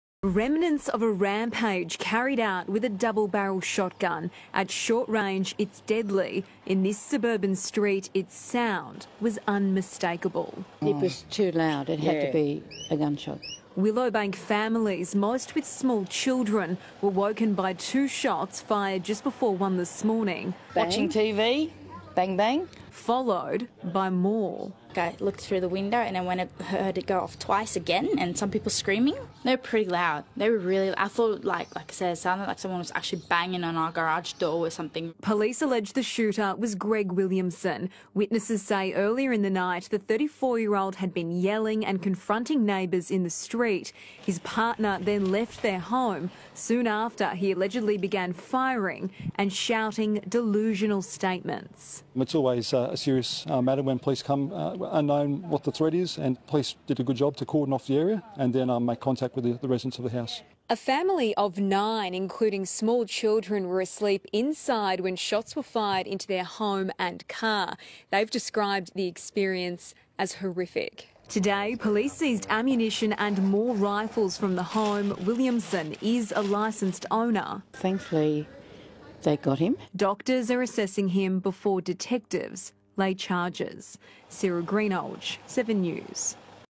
Shots Fired Into House/Cars - Willowbank, Ipswich (2017)
A man has been charged after firing shots into a house and cars on a suburban street in Ipswich.